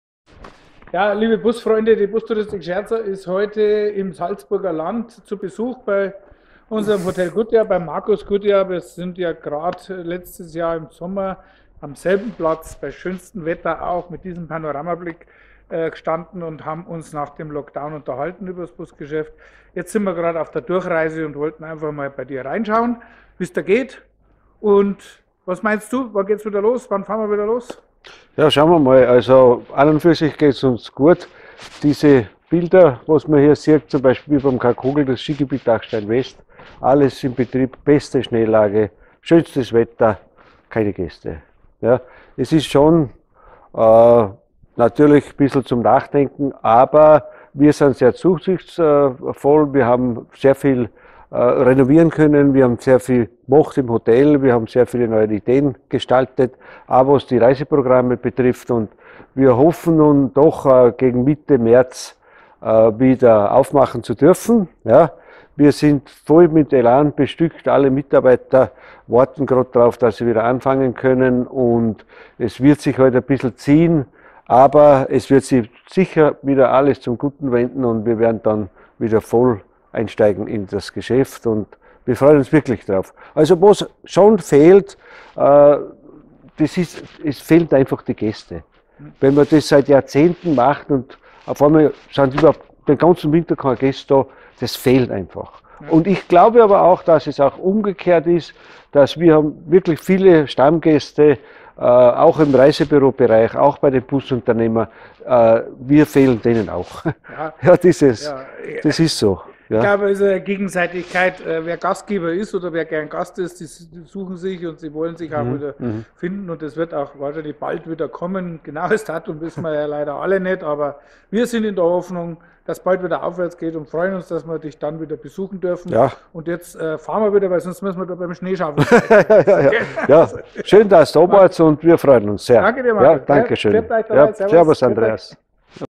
Zu Besuch bei Markus Gutjahr
*𝘞𝘦𝘳𝘣𝘶𝘯𝘨* Wir haben mal wieder einen Abstecher zu Markus Gutjahr gemacht... Trotz bestem Wetter natürlich auch hier keine Gäste, aber die reisefreie Zeit wurde gut genutzt👍 Das Hotel Gutjahr ist bereit für Besucher und hofft auf einen Eröffnungstermin im März🤗